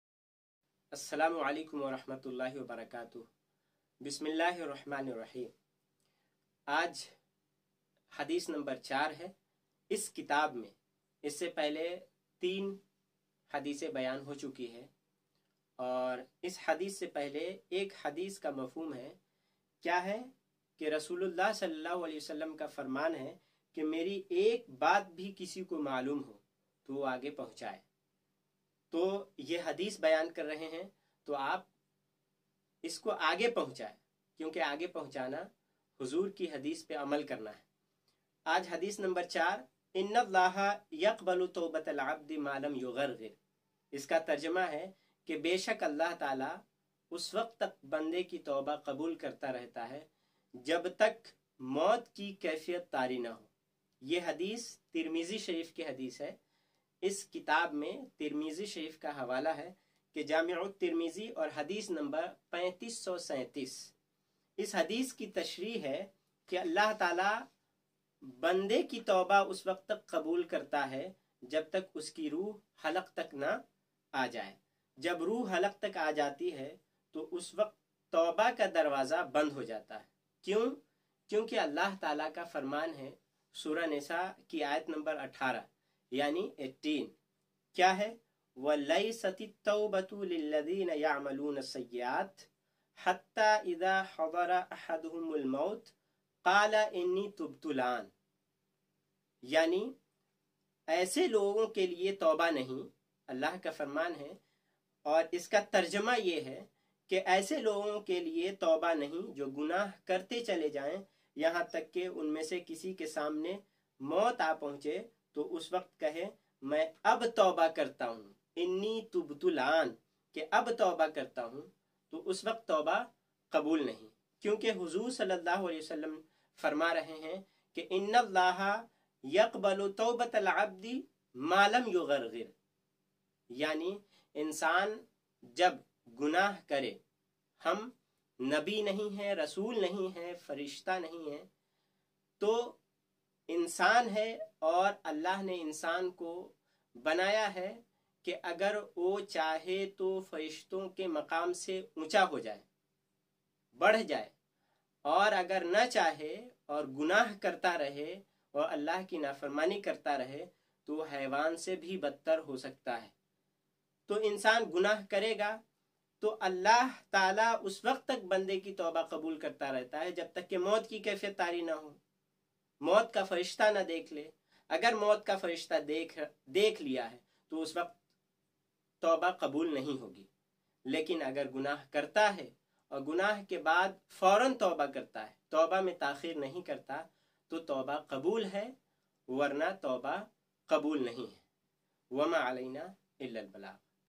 Rasulullah sallallahu alayhi wa sallam ke Sunharay irshadat Part 2
is video meñ hadith bayaan ki gai hai
jo naujawaan nasl ko sudhaar sakti hai